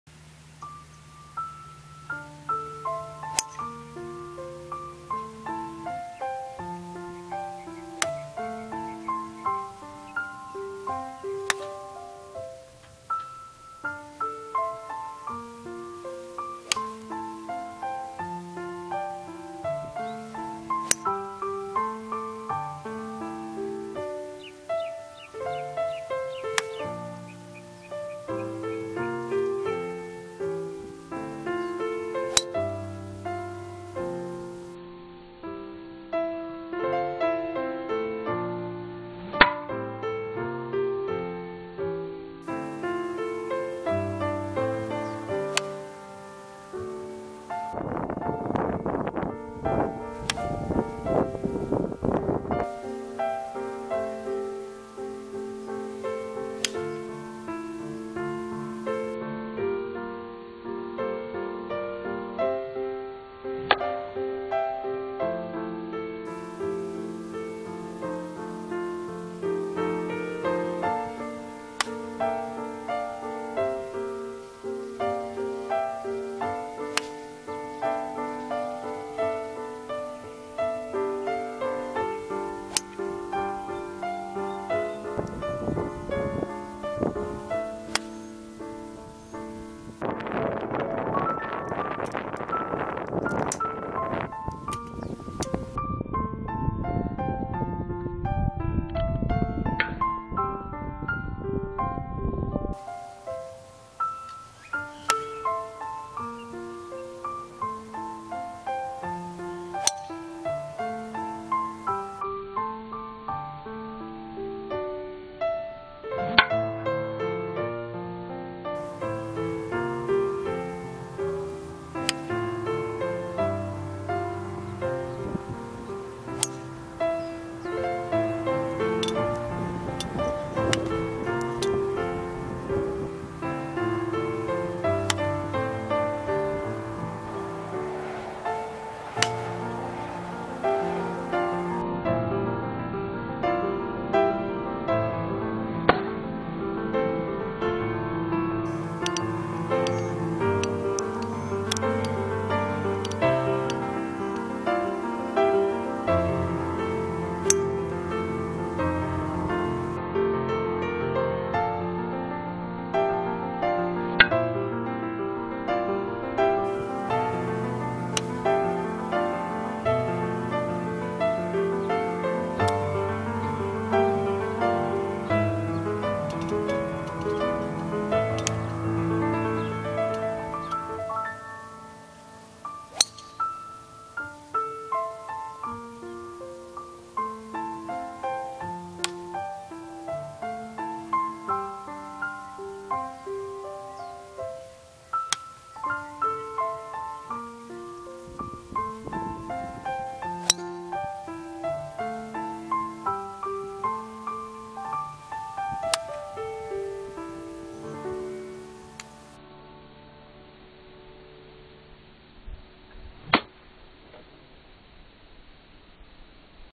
More element from George than Moe this time - Inspired by George Knudson & Moe Norman
George Knudson's set up method is more conventional than Moe. But he put the ball toward the toe of the club head...
Still need to work harder on the rhythm, Knudson's is much more effortless...